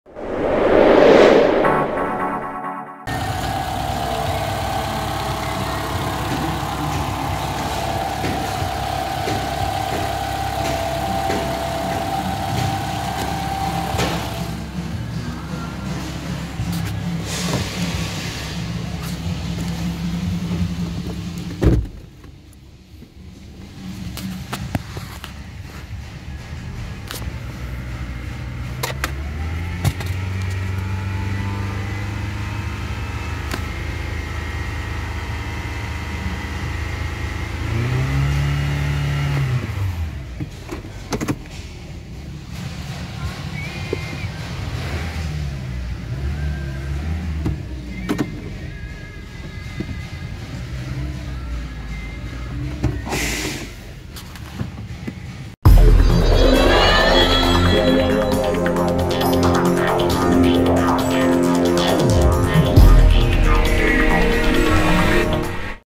WRECKING 2017 TOYOTA CAMRY 2.5L PETROL AUTOMATIC C35998
We are currently WRECKING 2017 TOYOTA CAMRY 2.5L PETROL AUTOMATIC C35998.